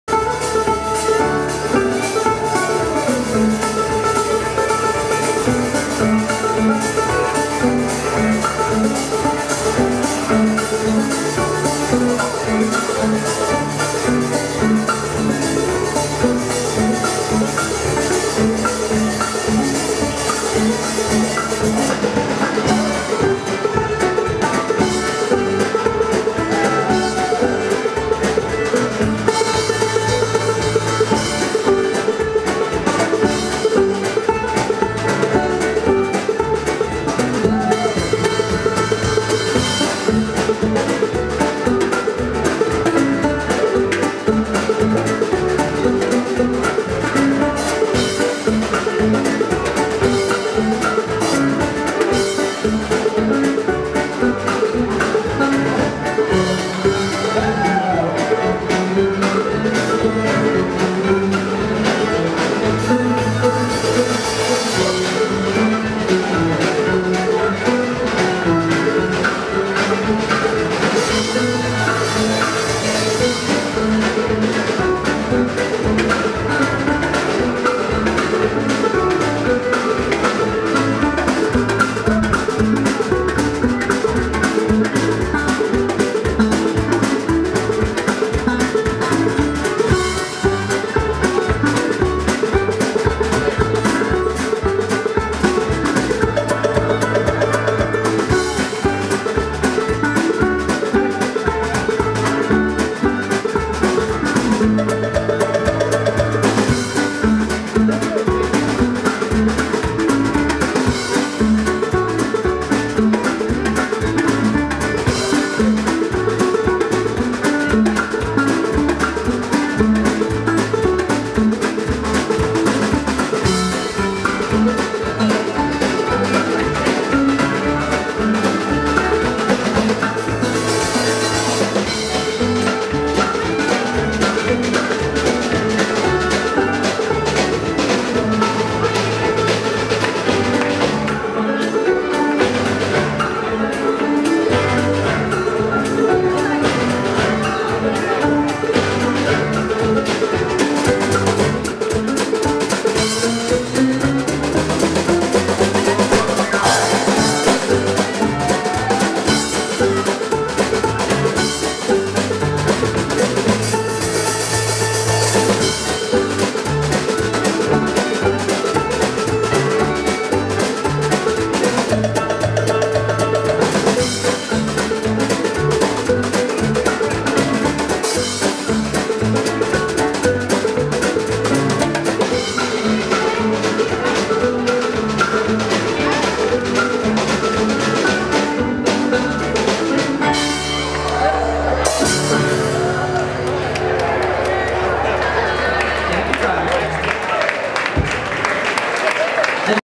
River Rendezvous 2016 - Take 2M
2M - Emily Rainbowface Abel prompting, as Hotpoint Stringband plays. On fiddle - Mark Burhans, on banjo - Hilarie Burhans, on keyboard - Marlene Shostak, on bass - Nick Weiland, and on percussion we have "Pokey" Hellenberg. How come this band is having so much fun? (This dance sequence is "Centrifugal Hey" by Gene Hubert.)